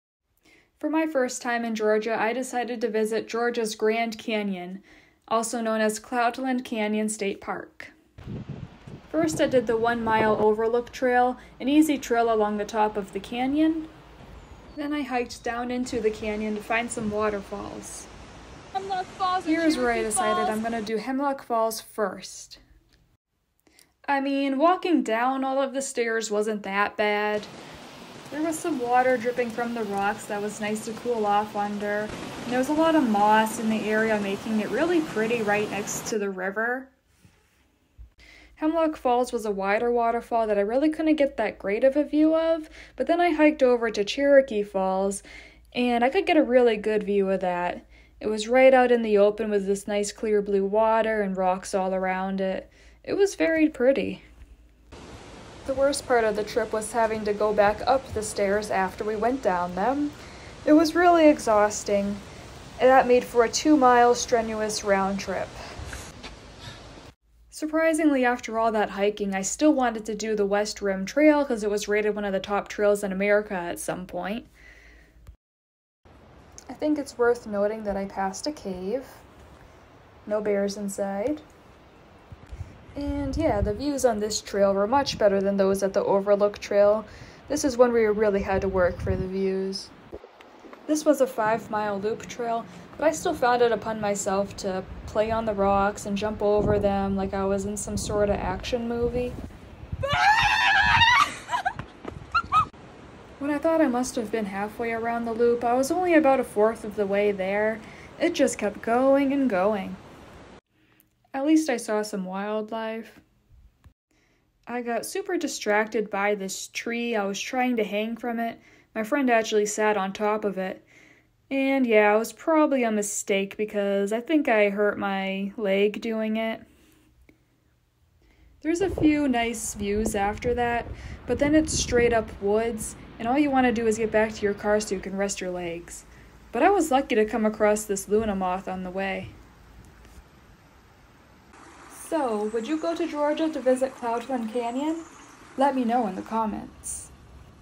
Cloudland Canyon State Park: 3 Best Trails
My adventure hiking the Overlook Trail, Waterfalls Trail, and West Rim Trail at Cloudland Canyon State Park in Georgia